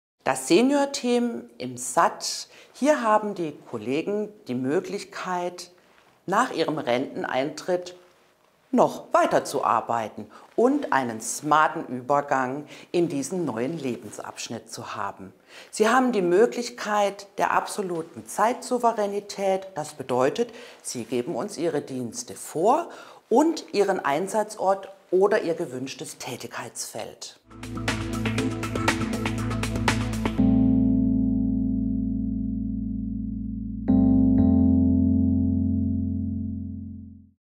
Was ist das Seniorteam im SAT? - Arbeiten im Krankenhaus | Asklepios
Hast du dich auch schon einmal gefragt, wie du auch nach dem Renteneintritt deine wertvolle Erfahrung in der Pflege weitergeben könntest? Das Seniorteam des SAT-Programms bietet dir die Chance, aktiv zu bleiben und nach eigenen Wünschen und Zeitvorstellungen zu arbeiten. Manuela Fischer-Willmann, Bereichsleitung Pflege am Asklepios Klinikum Harburg erklärt, was das Seniorteam im SAT ist und wie du auch nach Renteneintritt die Zukunft der Pflege mitgestalten kannst.